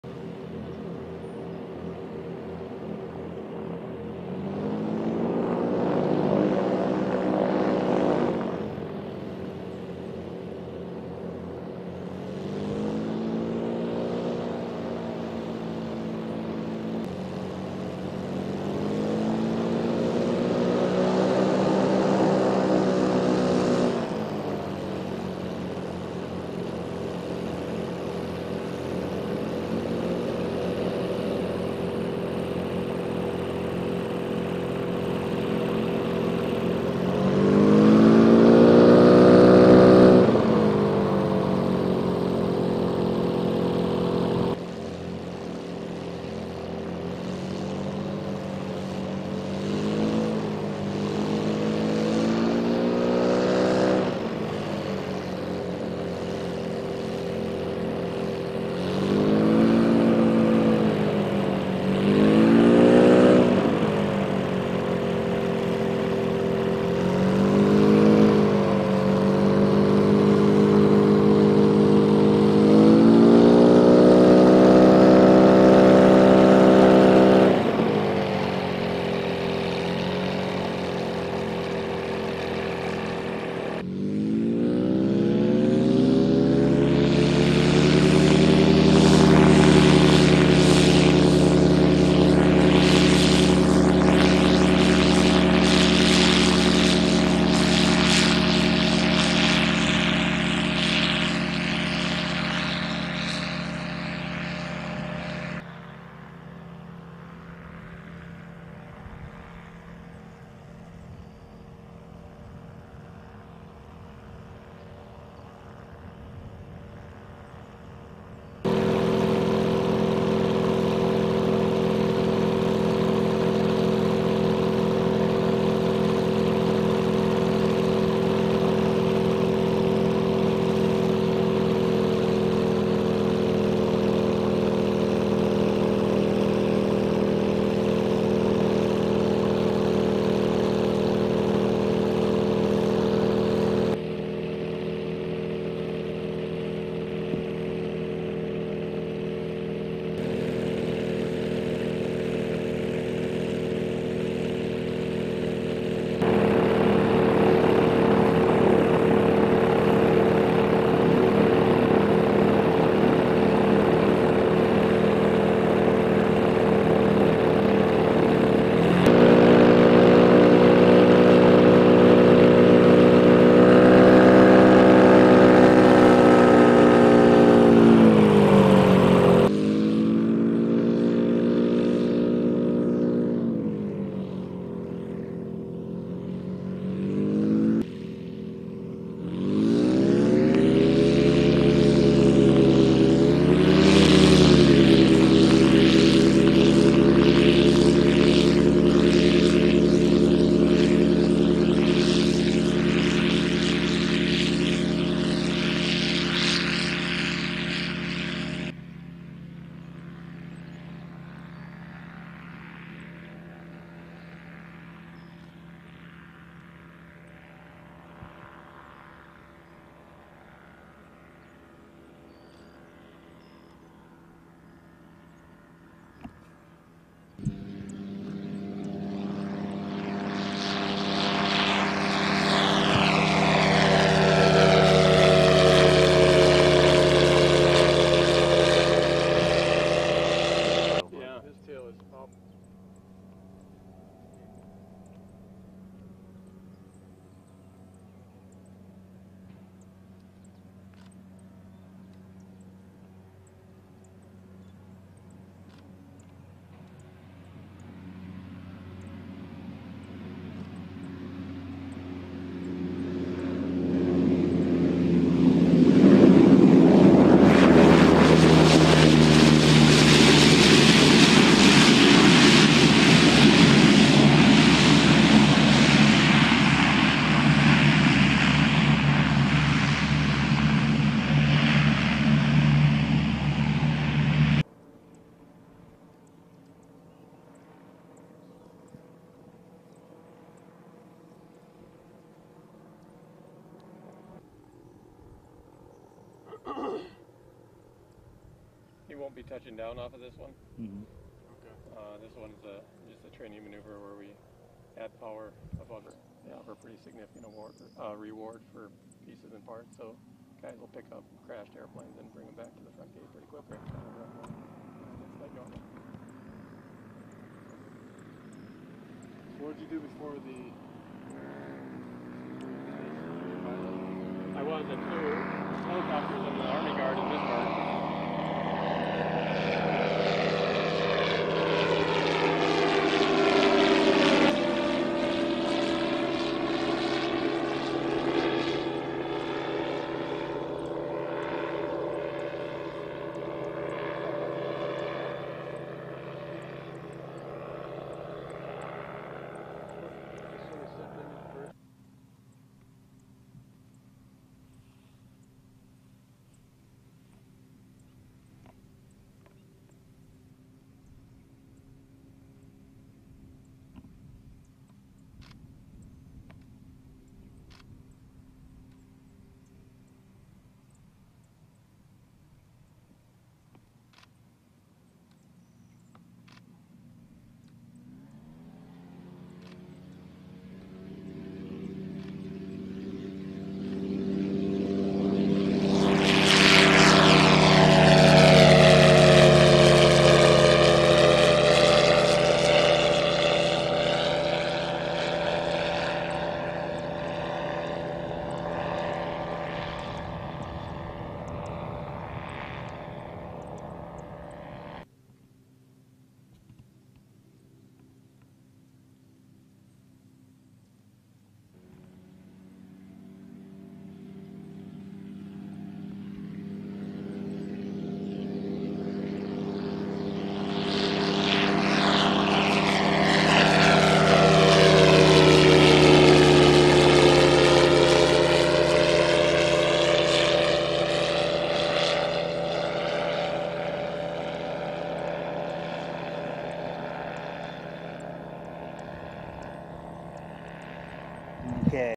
US MIlitary's Eye in the Sky: MQ-1 Predator Drone Takeoff and Land
119th Wing, North Dakota Air National Guard, MQ-1 Predators takeoff and land at Grand Forks Air Force Base as part of the Launch and Recovery Element. Credits: TSgt. Michael Knodle | 119th Wing Public Affairs North Dakota Air National Guard | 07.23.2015

AiirSource℠ covers events and missions from the Army, Navy, Marine Corps, Air Force, and Coast Guard.